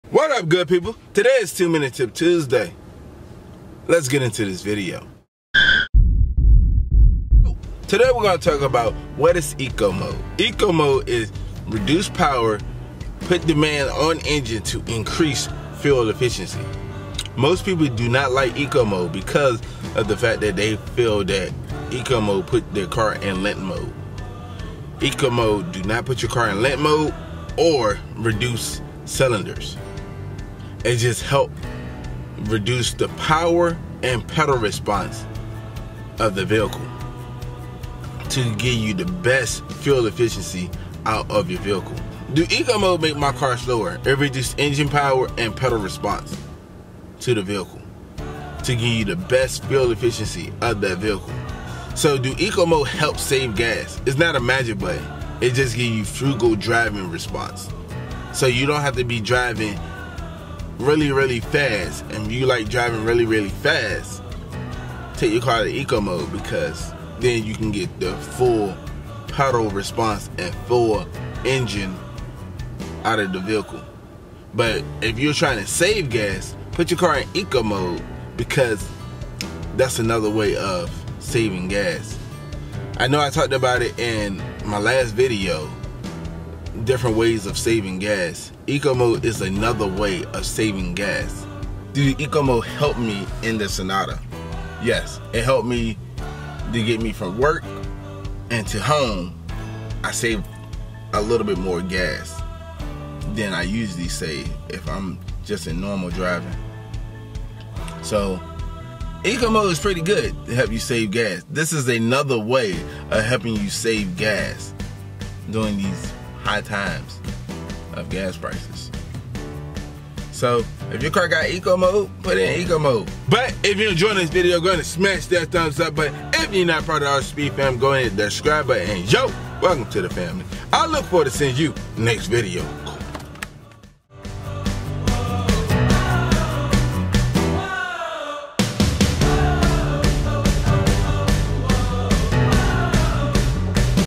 Does Eco Mode Really Save Gas?
The Eco mode (or Econ on some vehicles) is a fuel-saving feature offered by several manufacturers that instructs the engine computer to shift into a program that maximizes efficiency over performance

Eco mode is great when you're driving on the highway because the car is mostly cruising. When you're in traffic, you've got longer idle time, less acceleration so the engine switching off (Start/Stop) is handy but the air conditioning is turned down too